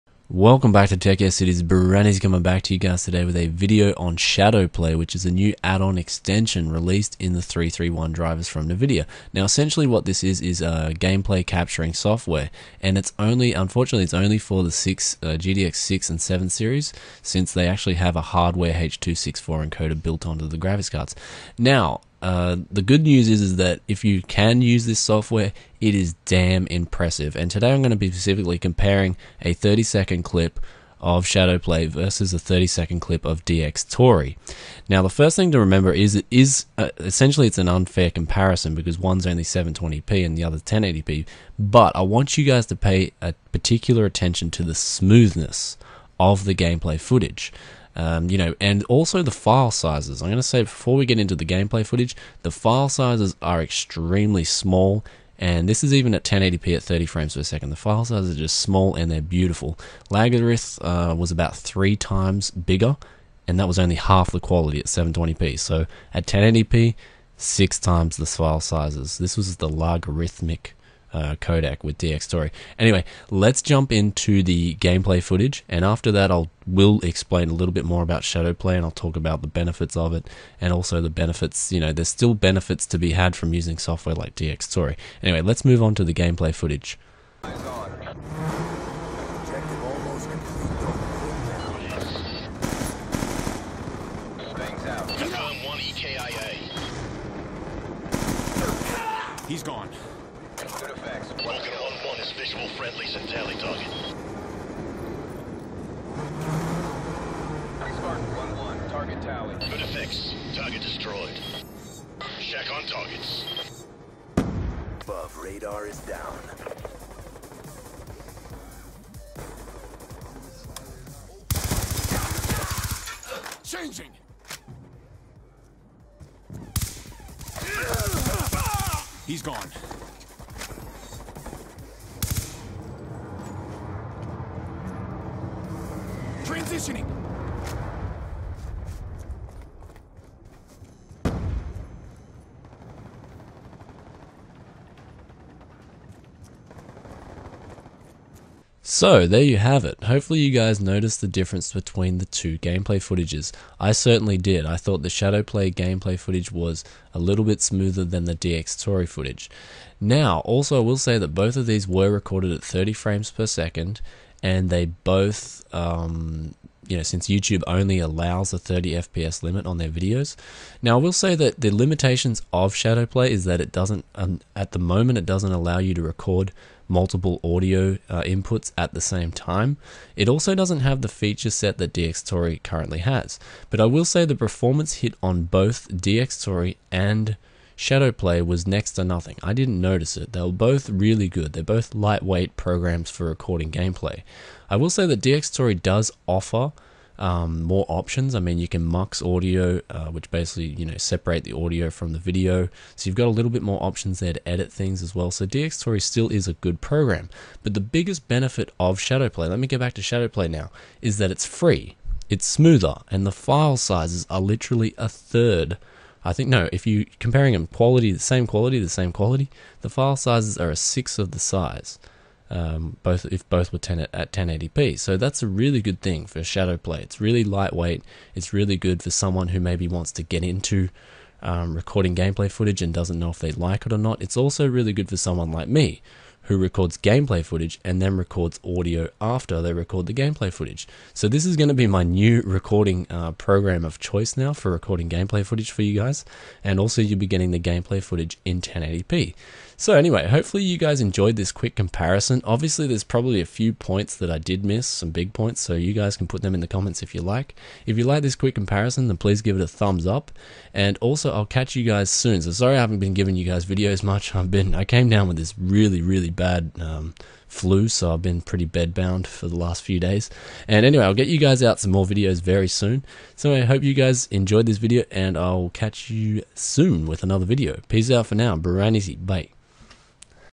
Nvidia SHADOWPLAY Review Plus Quick Comparison Vs. DXTory
Hey there welcome back to Tech YES City, sorry for the delays on bringing you guys a video, I have been sick lately with the plague going around here.

Also important to note that I had to install the shadowplay 331.65 drivers and software 2 times (as it didn't work the first time).

Nvidia recently released their recording software called shadow play and it is damn impressive, the performance hit was next to nothing (also the same with dxtory) however the file sizes and recordings were simply amazing.  As youtube only allows 30fps limit this comparison will be a little bit dull, but let me say that is a great addition and the main fact that it is free is really awesome.

Vs FRAPS I don't even want to go there as I stopped using fraps when I was getting file sizes that were bigger than my harddrives.